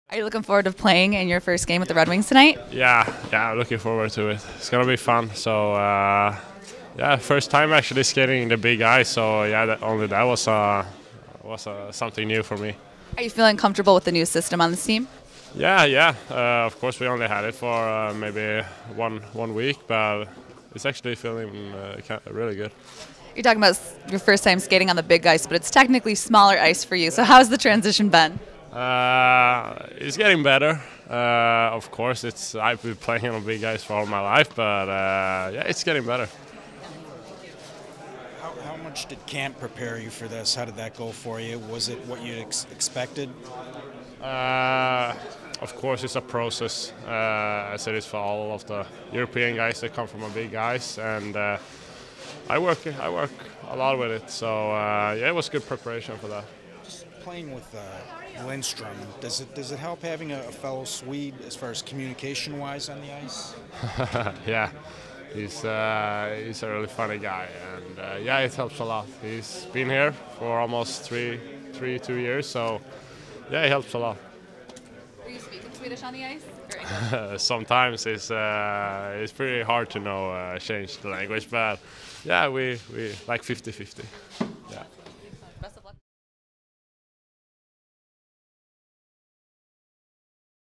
Simon Edvinsson ahead of Chicago vs. Detroit
Simon Edvinsson speaks with the media follow morning skate.

The Red Wings take on the Blackhawks in their second preseason game of the season at 7:30pm at Little Caesars Arena.